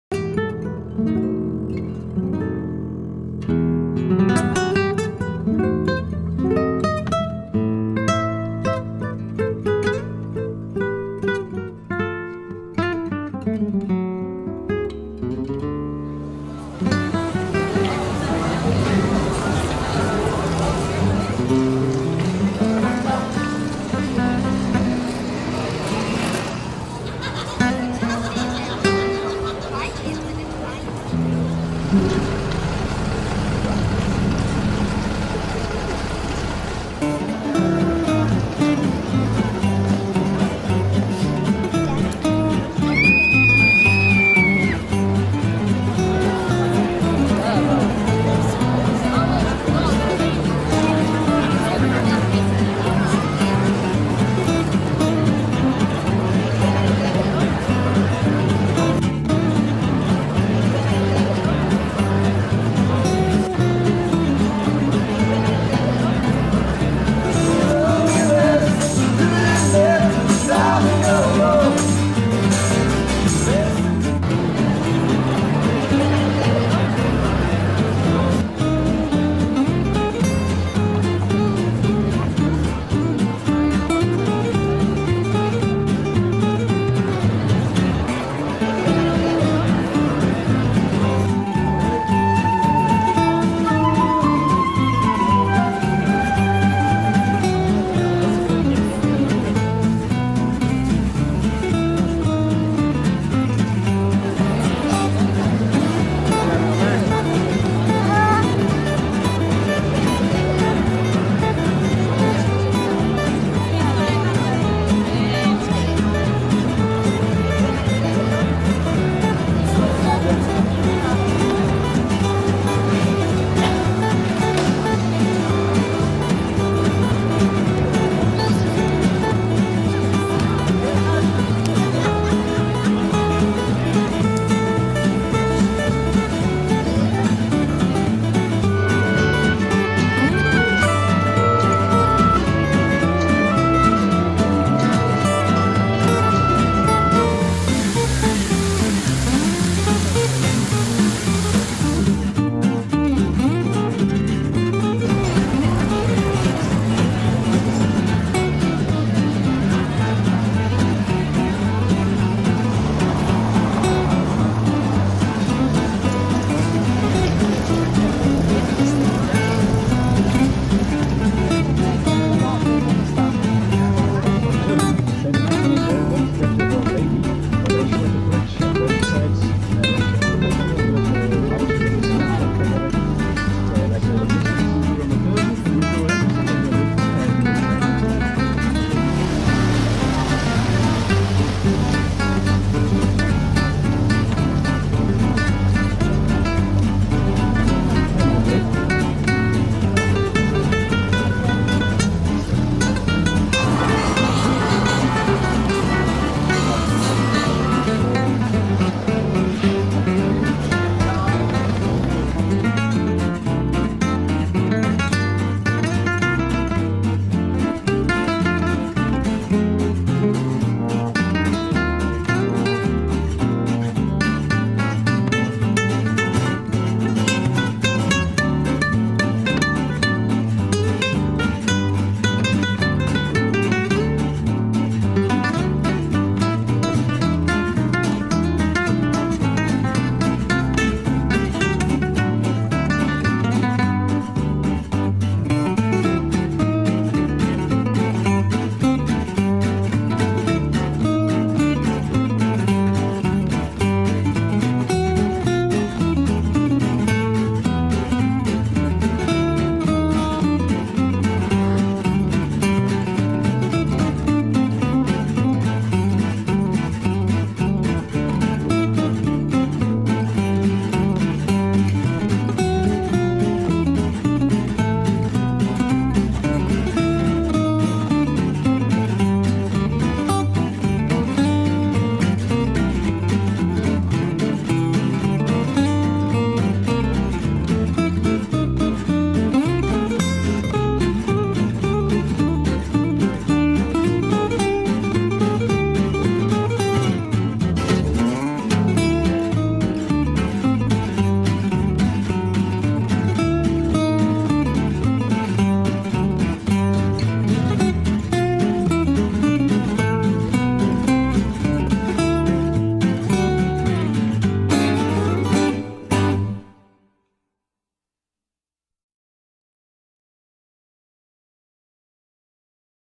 SUMMERTIME+IN+BELGIUM HD
This video was shot in the summer of the crowning of the new king of Belgium - Philippe I
It was shot in Brussels, Brugge, Namur, Dinant, Durbuy and Annevoie in july 2013.